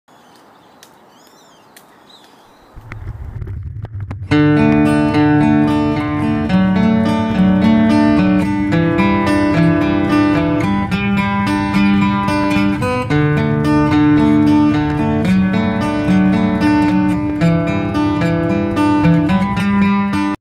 Foresters Trailer
Hello guys this is my channel trailer. This is namsan park in south korea when it is snowing!